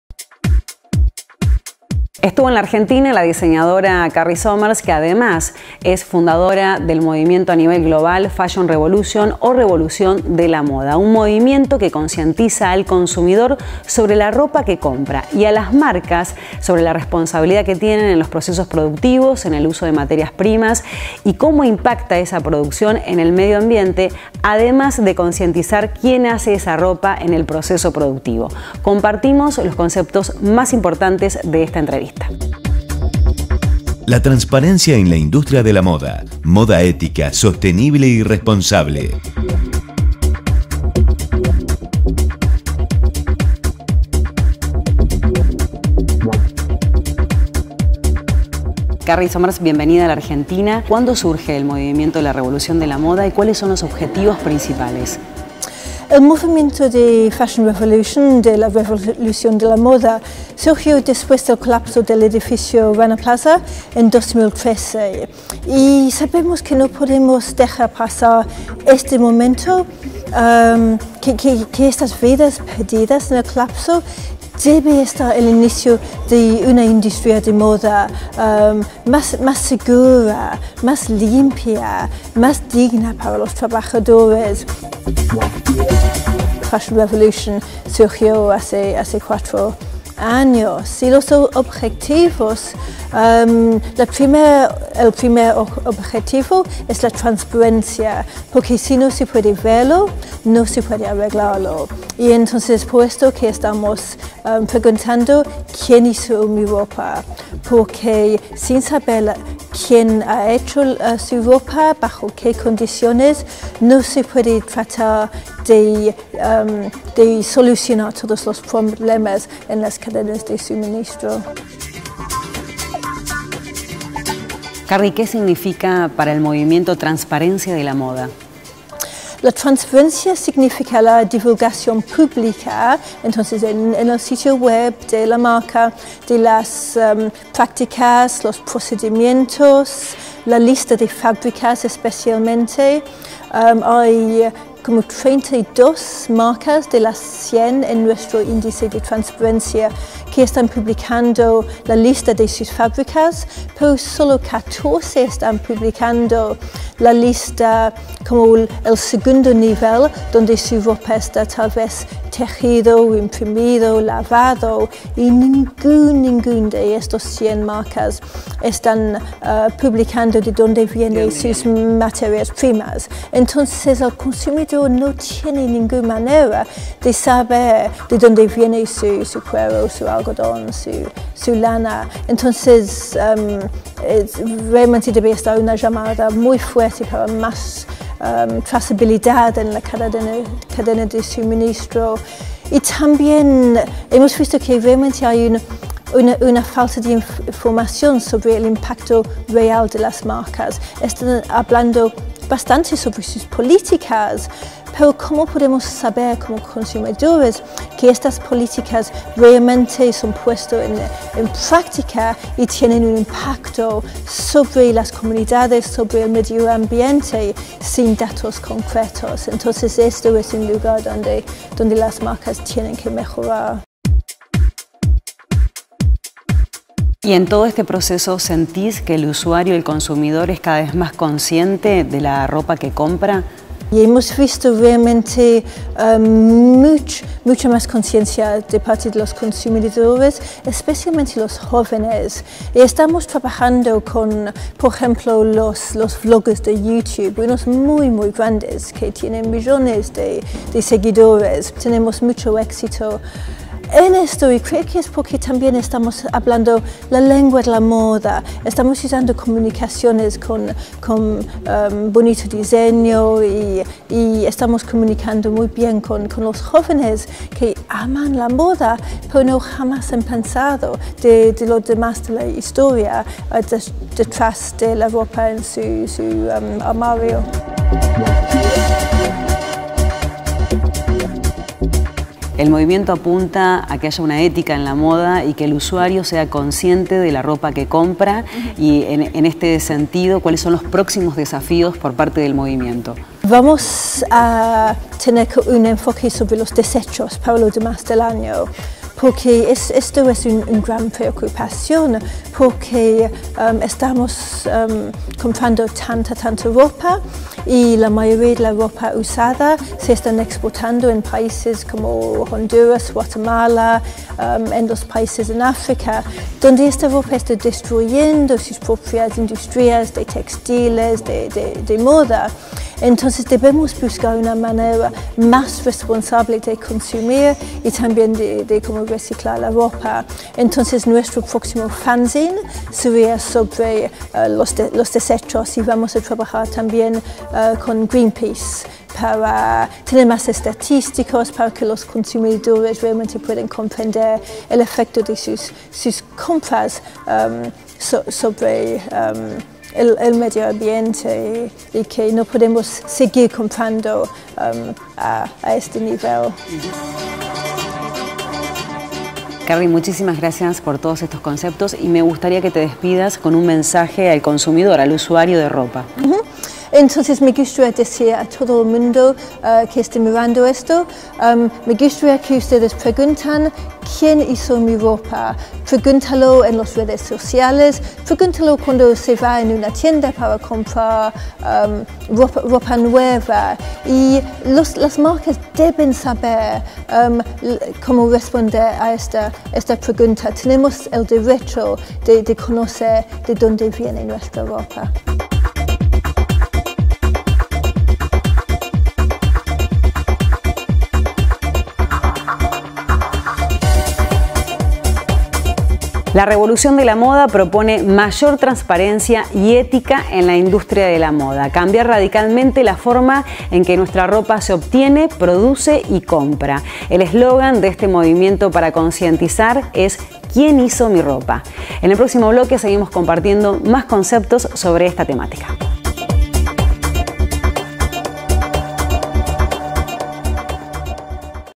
Carry Somers TV interview. Plan Textil, Argentina
Entrevista con Carry Somers sobre la transparencia en la industria de moda.